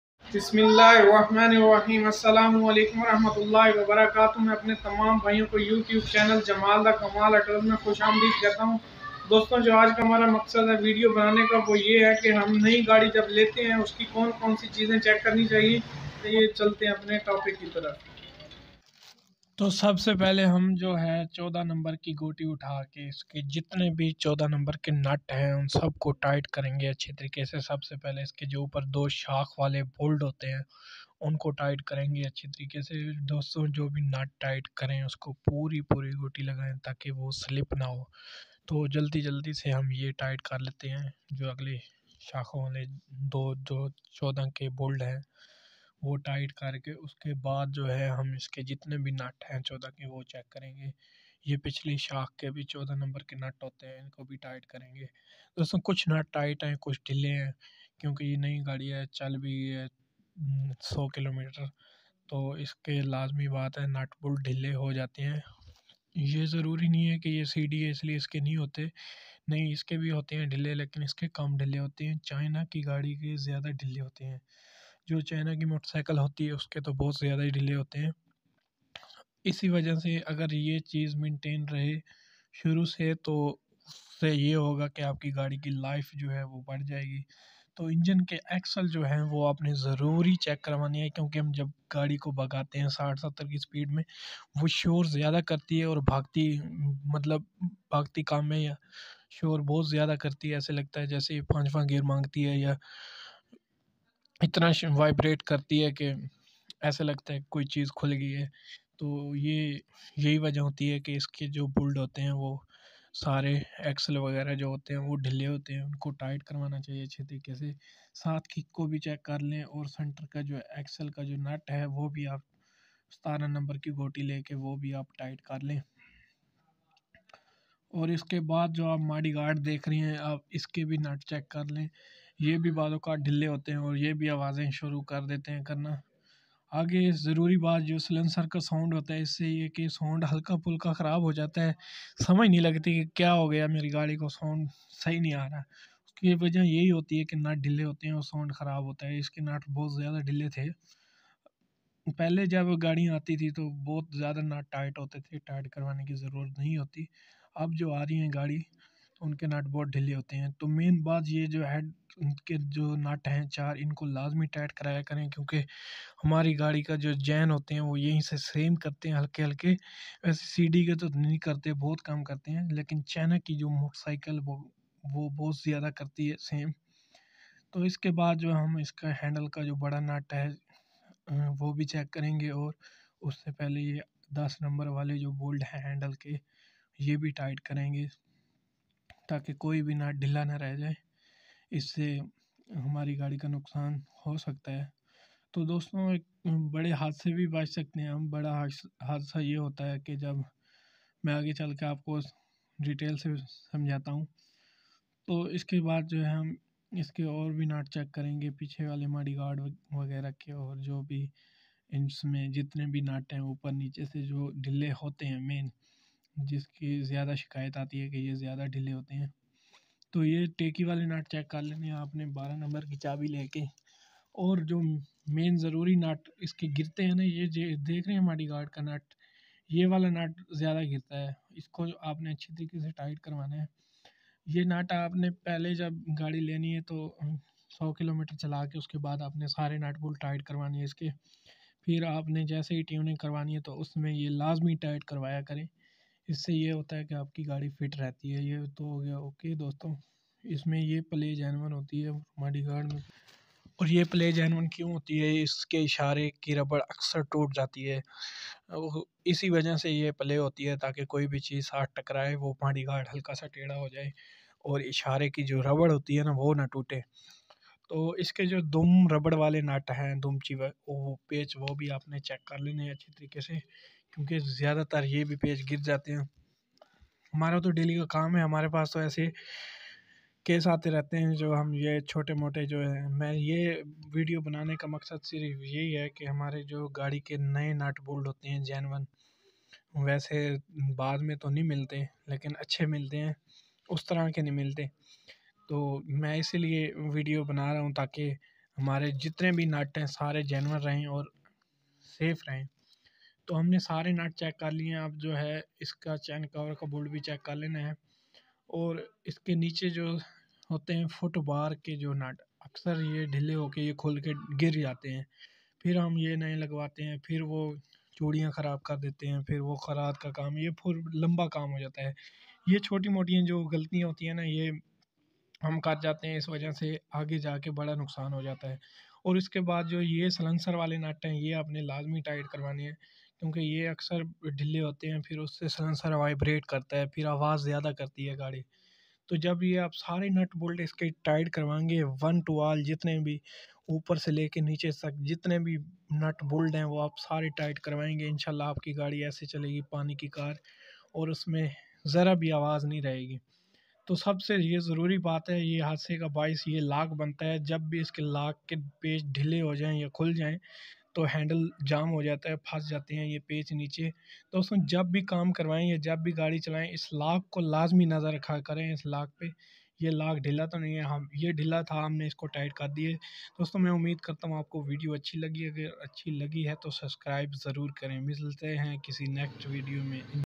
CD 70cc 2021 model mein kaun kaun sa kam Karana chahie aur kya kya chij zaroori hai
2001 model CD software mein kaun kaun si meeting check karvani chahie kaun kaun sa kam Karana chahie aur kya kya chij Jaruri check Karni chahie apni bike ki father aap khud Karen